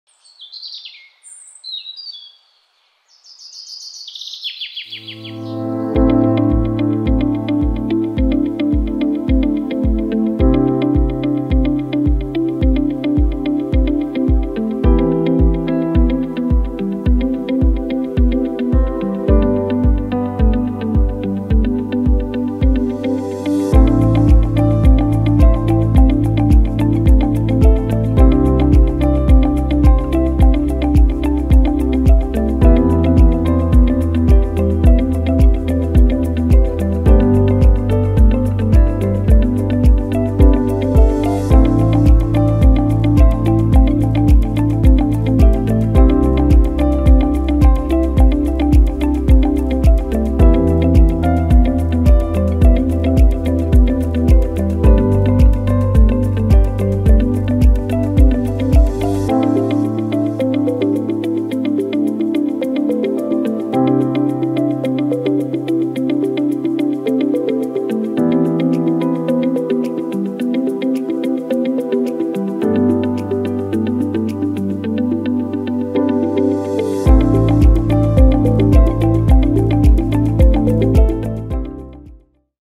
Kowa SV II Binoculars - In Touch with Nature
The Kowa SV II range benefits from the same great optics in the original SV binoculars and continue to offer an excellent price/performance ratio with multi-coated lenses, KR coating, lightweight body and sleek ergonomics that made the original line-up so popular. The SV II series features a cosmetic upgrade to a new stylish green body colour for the our 8x32, 10x32, 8x42, 10x42, 10x50 and 12x50 models - Kowa SV II - in touch with nature.